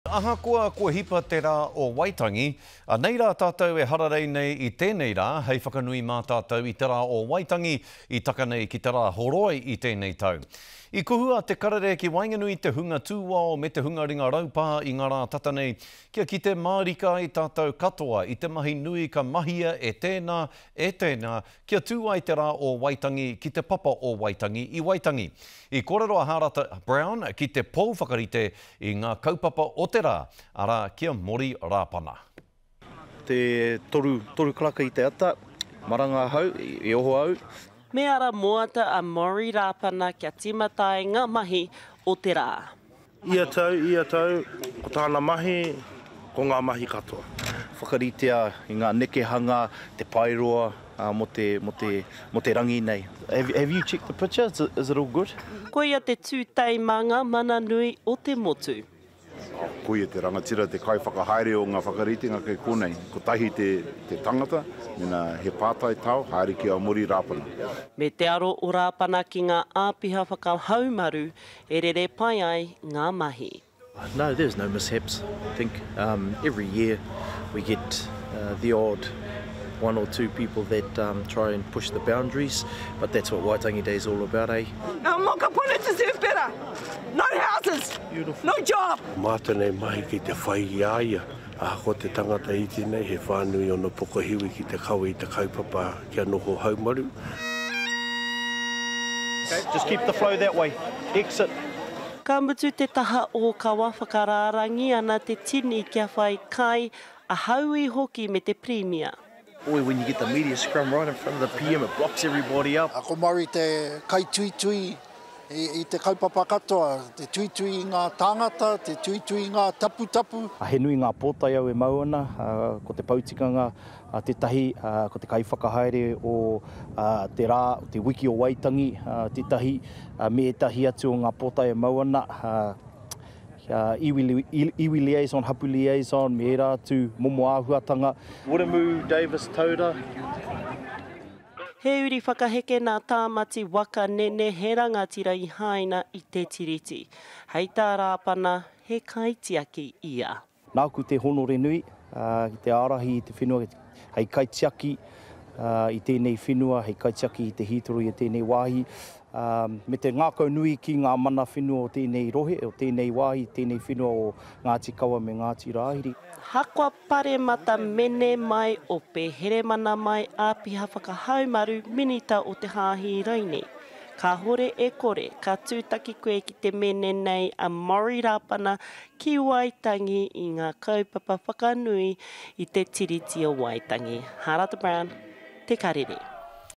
Te Karere meets the man behind the Waitangi Day celebrations
Kumara may not speak of their sweetness, but some people do so much, that others speak their praises.